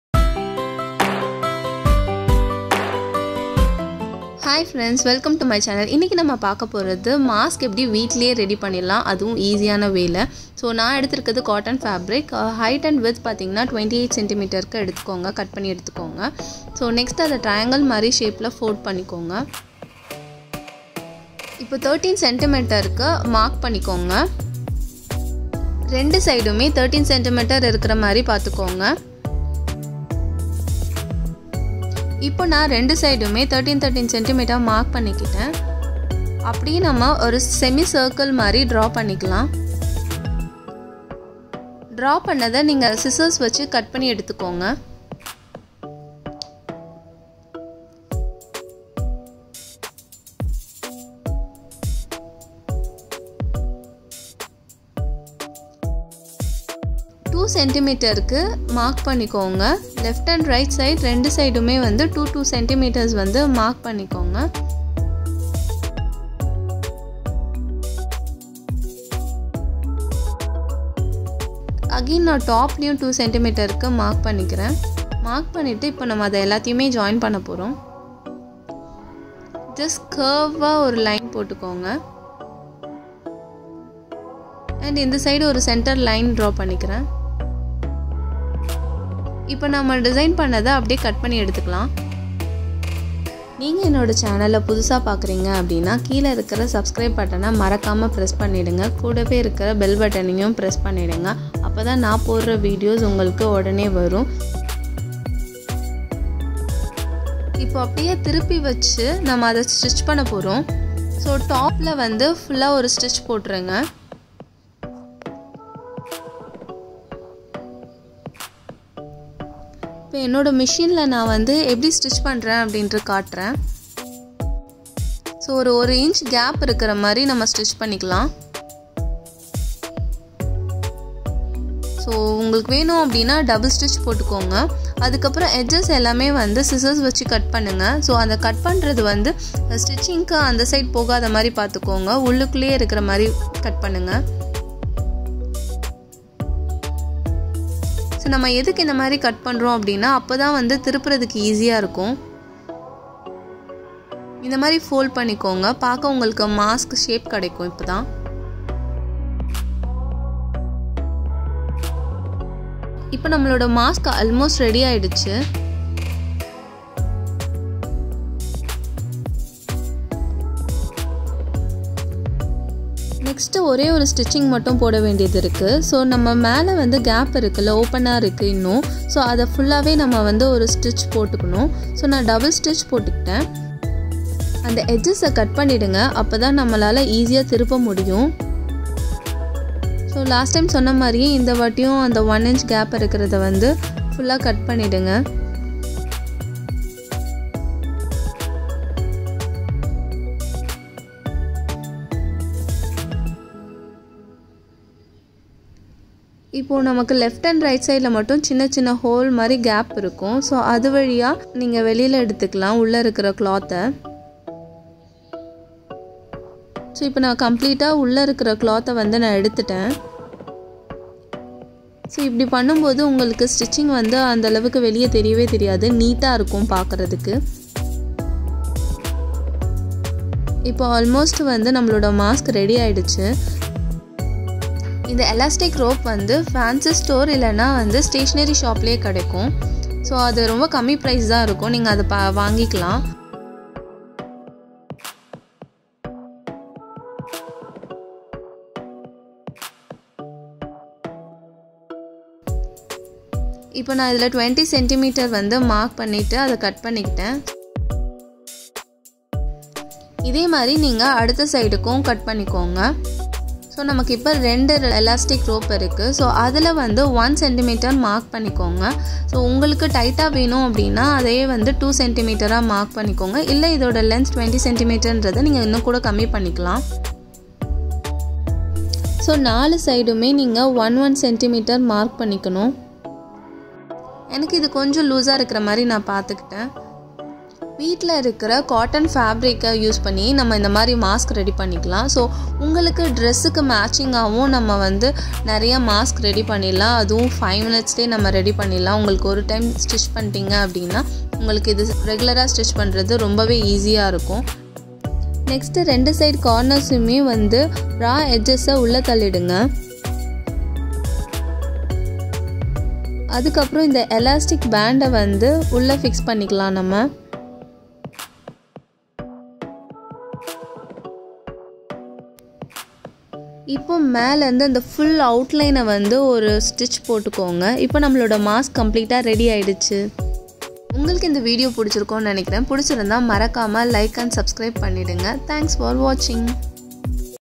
How to stitch Mask in 5 mins!? | Easy Mask stitching for corona in Tamil | DIY
How to stitch Mask in 5 mins!? | Easy Mask stitching for corona | DIY | Stitching tutorial |Cotton Fabric | Tamil video